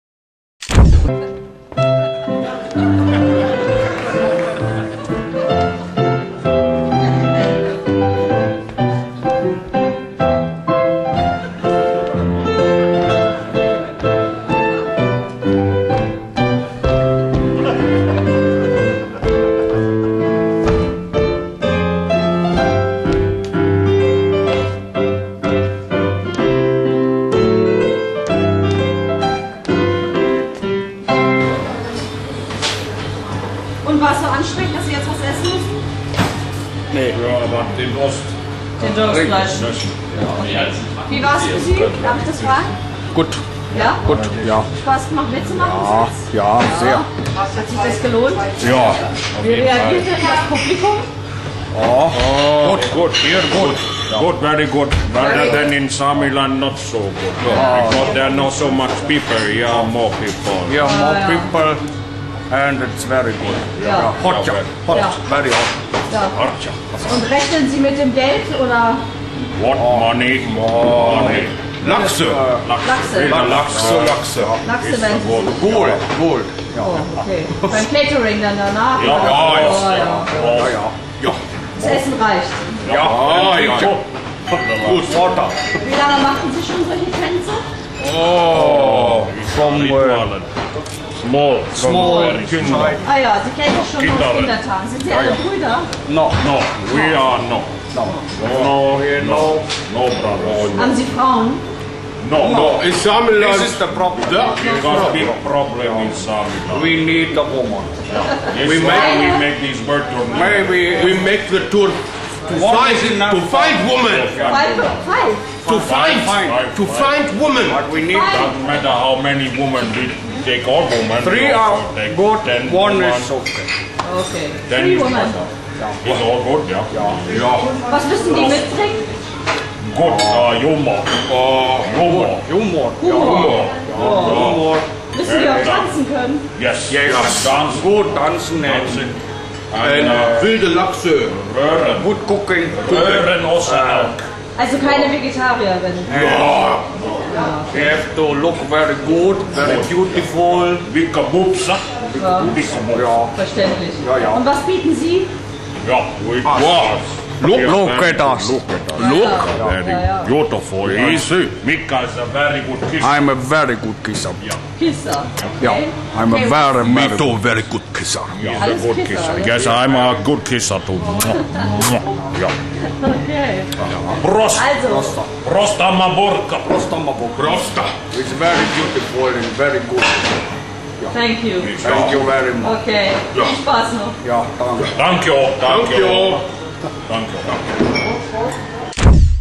Nordkvark good Kisser
Oben wir unten, vorne wie hinten, on stage wie backstage ... die Nordkvark. Mit freundlicher Genehmigung von good kisser.